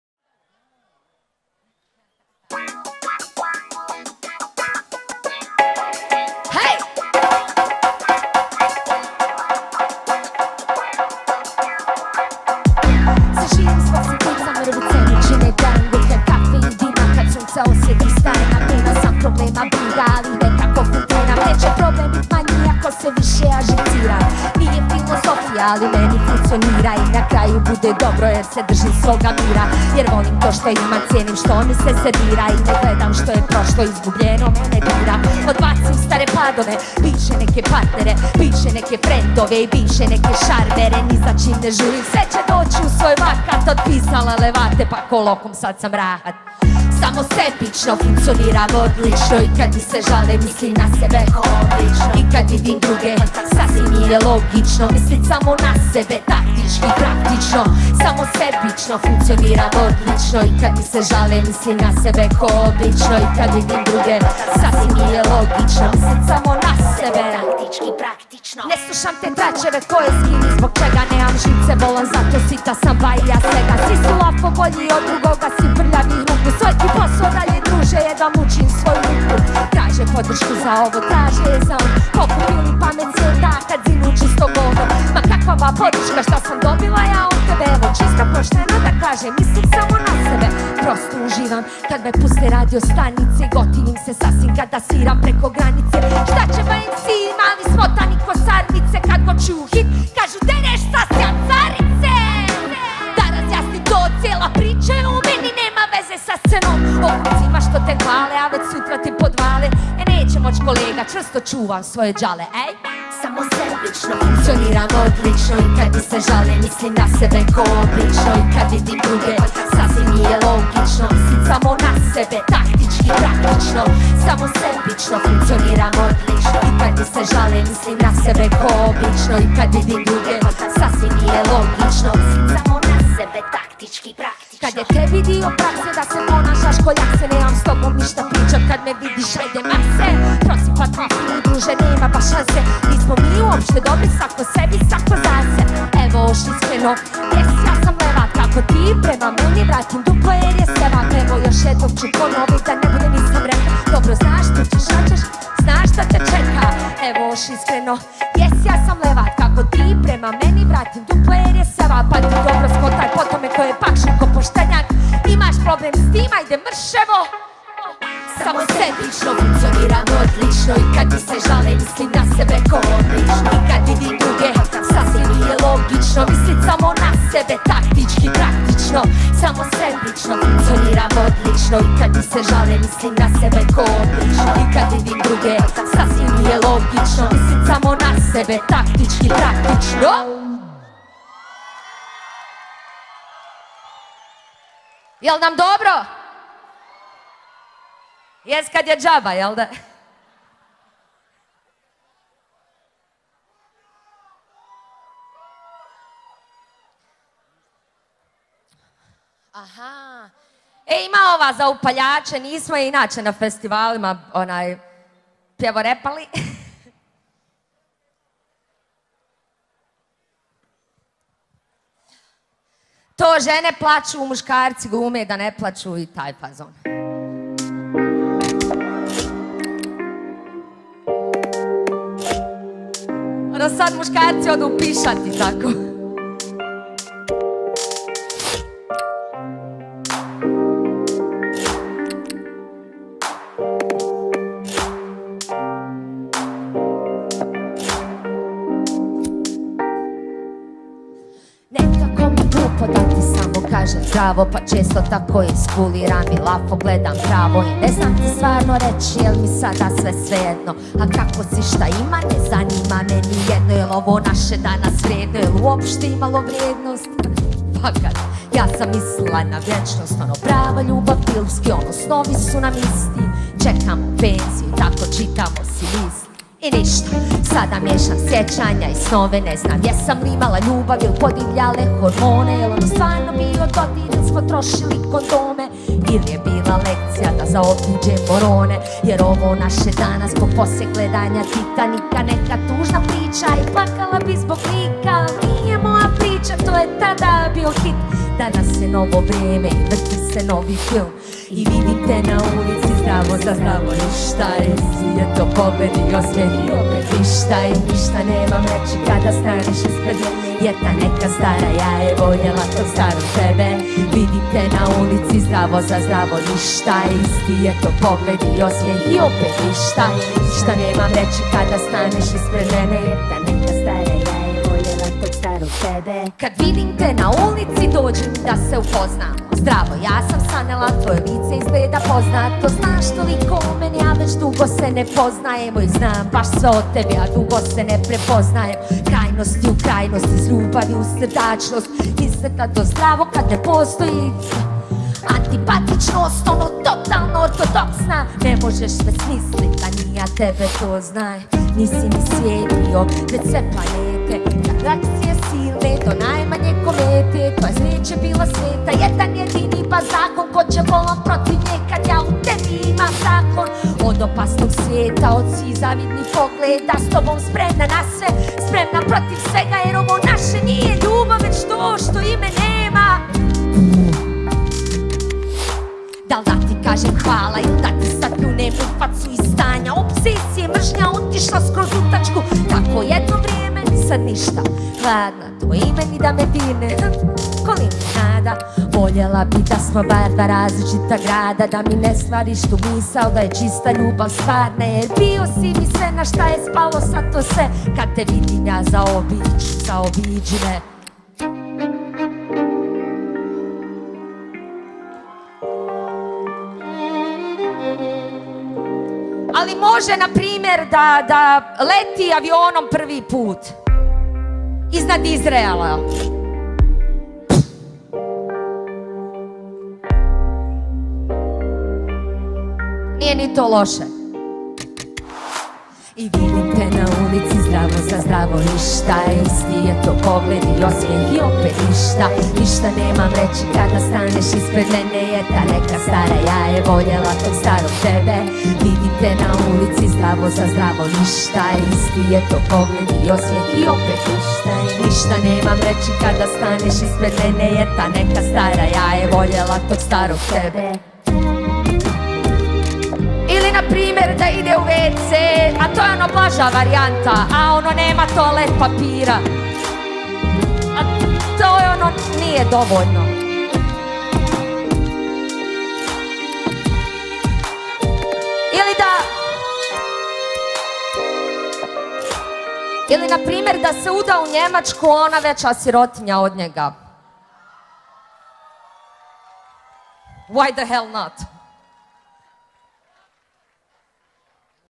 SASSJA Live Kaleidoskop 2017 (3)
Dio atmosfere sa nastupa SASSJE na 9.međunarodnom festivalu umjetnosti mladih KALEIDOSKOP 2017 Tuzla u organizaciji Grada Tuzla. Sound&Light - Imperio , Video- Pro Vision , izvršni realizator JKP Pannonica Tuzla, Subota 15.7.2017.g